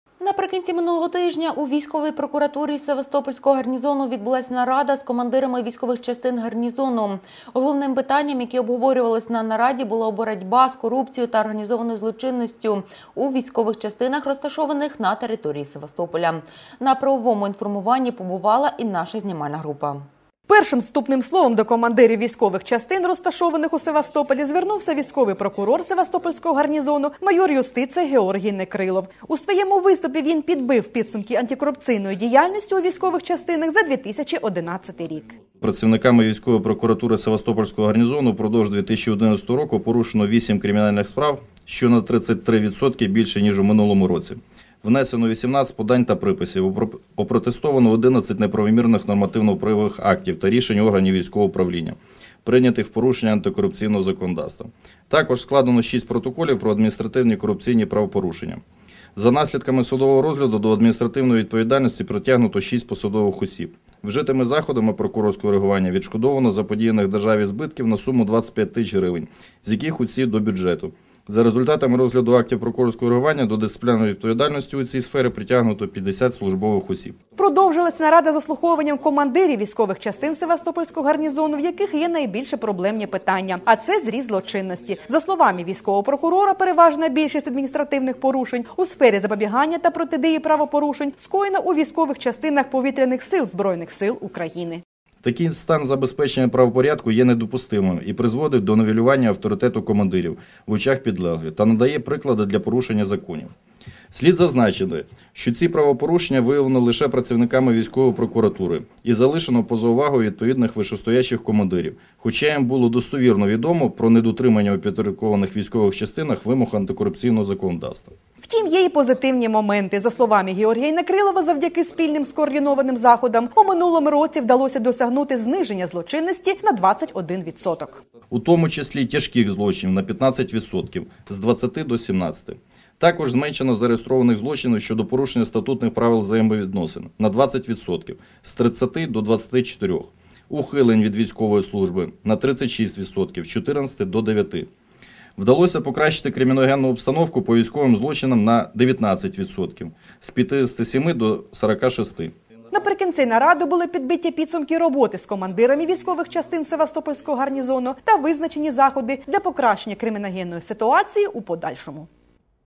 НАРАДА У ПРОКУРАТУРІ
Наприкінці минулого тижня у військовій прокуратурі Севастопольського гарнізону відбулась нарада з командирами військових частин гарнізону. Головним питанням, яке обговорювалось на нараді , було боротьба з корупцією  та організованою злочинністю у військових частинах розташованих на території  Севастополя